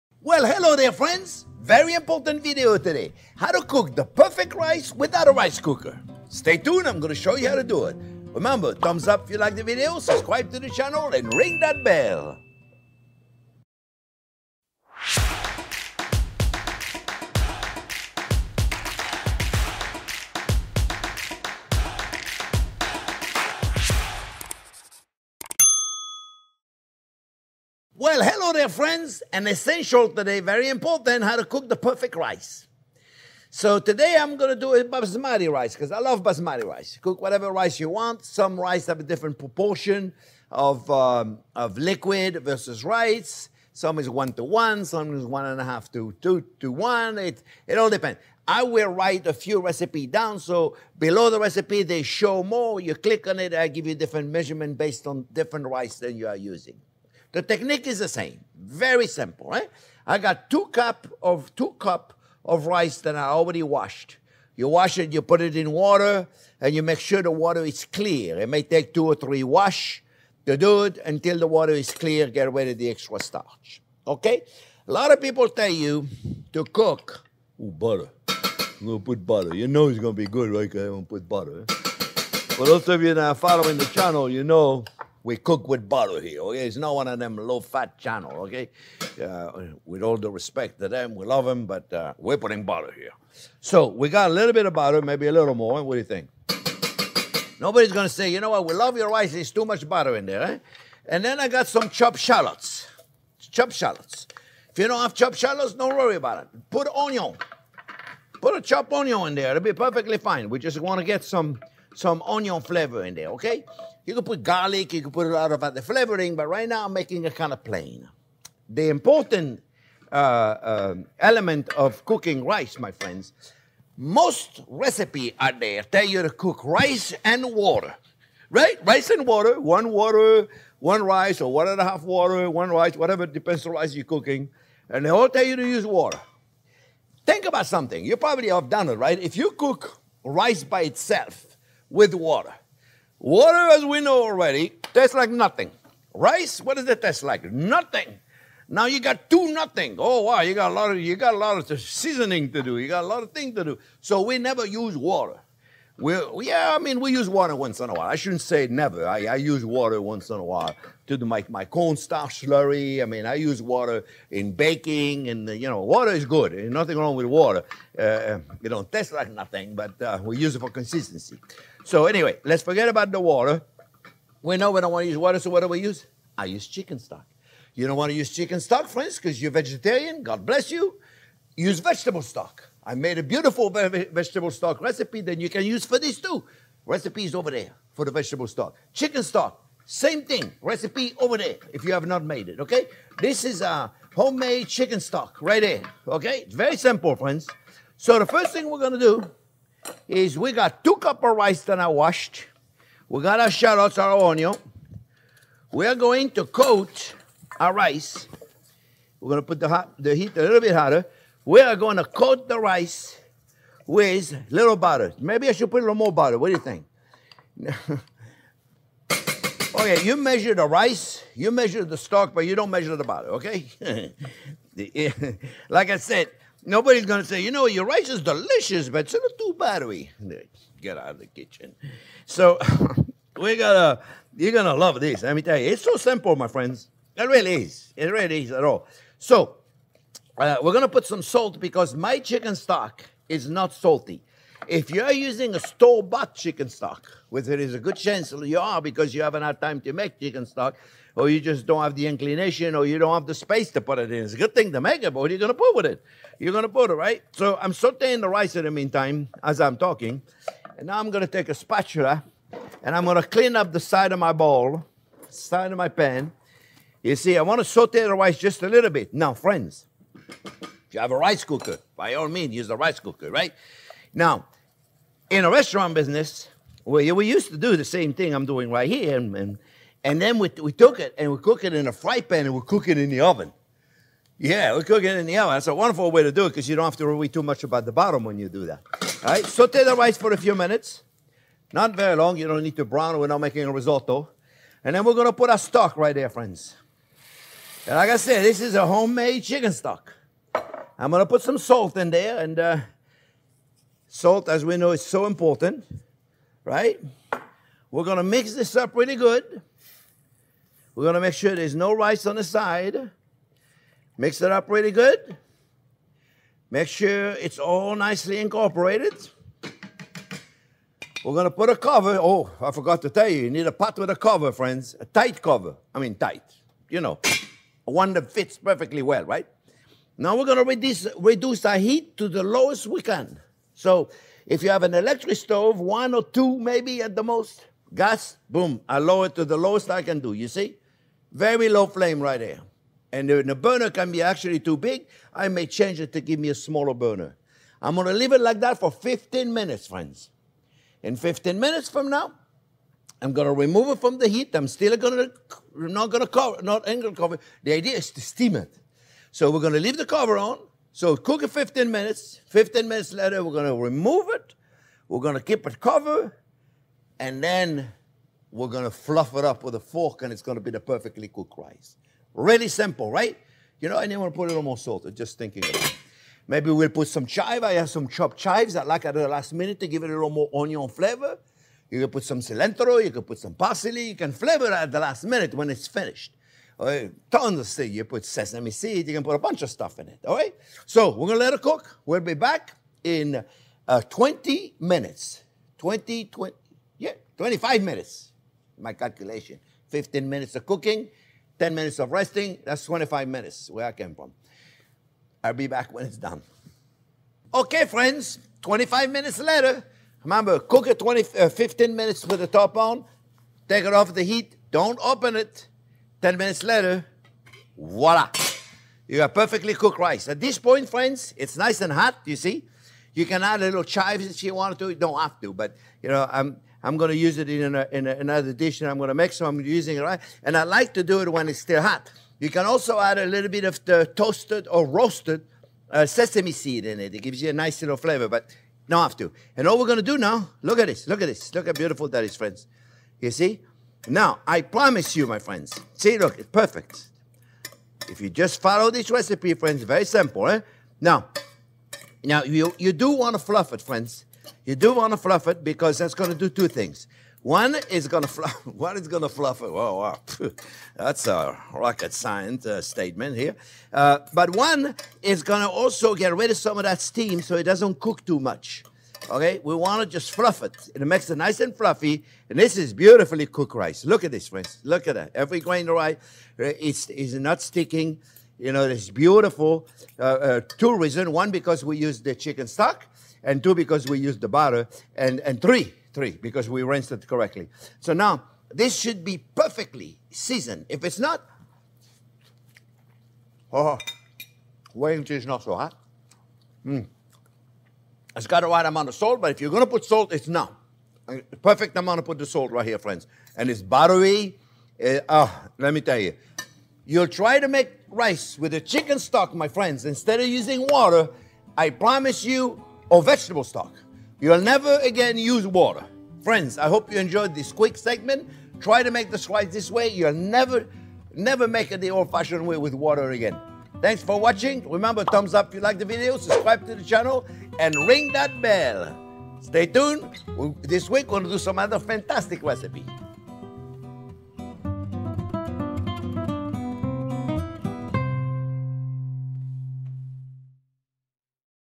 Perfect Rice Recipe | Chef Jean-Pierre
Hello There Friends, Today I’m going to show you how to make the perfect rice! A super way to make them with switching out the tasteless water for something else.